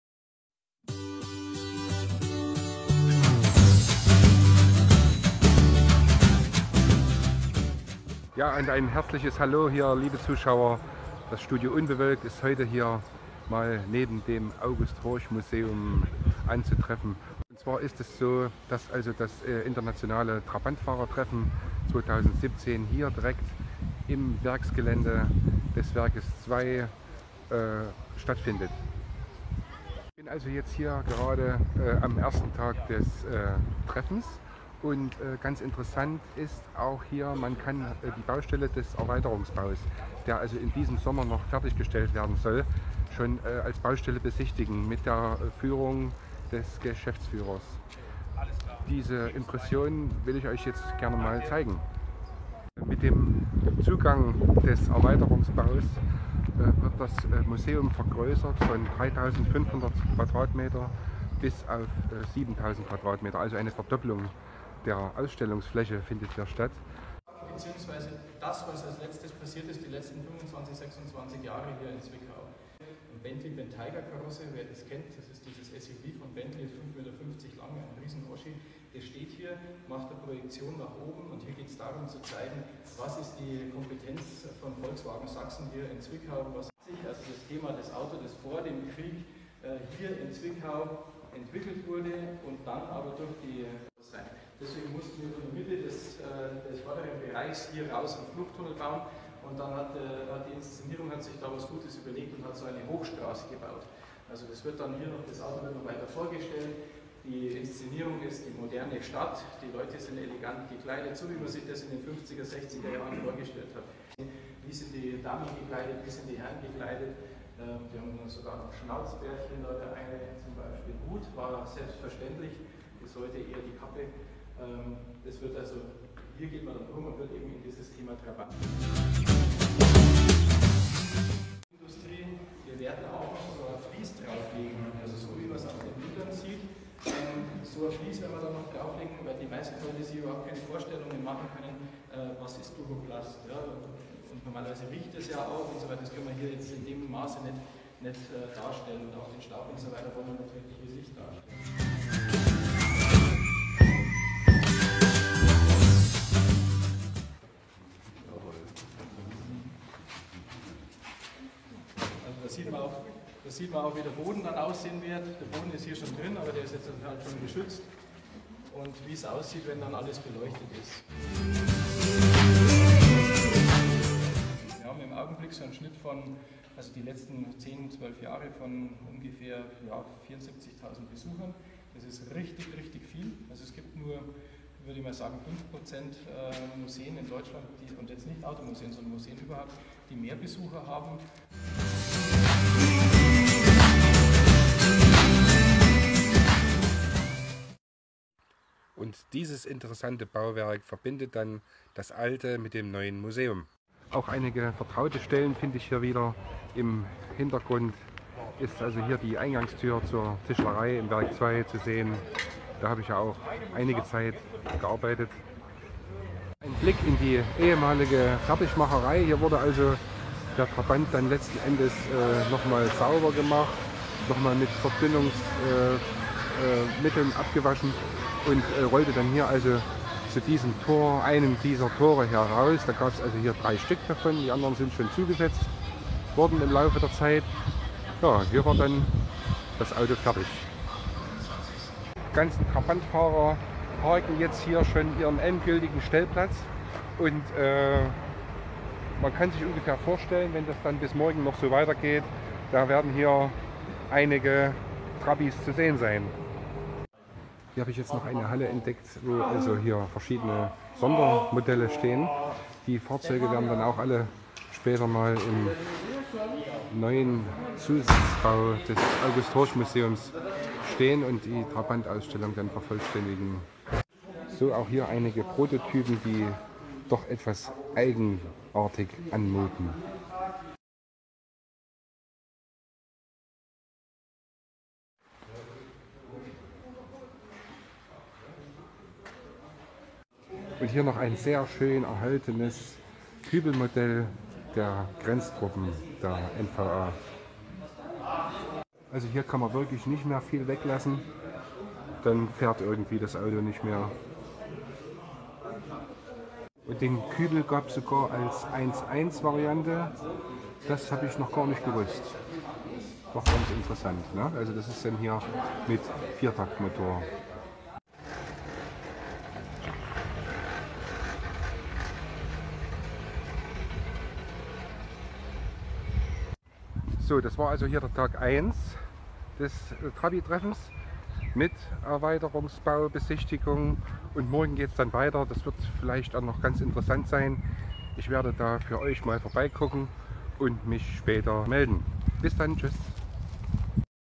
Trabant Treffen 2017 Studio unbewoelkt
Im Jahre 2017 findet das internationale trabantfahrer Treffen in Zwickau im ehemaligen Werksgelände des VEB Sachsenring Werk II statt dies ist die Geburtsstätte eines jeden Trabants.
Interessant ist auch das zum gleichen Zeitpunkt ein großer Erweiterungsbau das Museum von 3500 auf 7000 Quadratmeter anwachsen lässt hier kann auch die Baustelle als Schaustelle benutzt werden.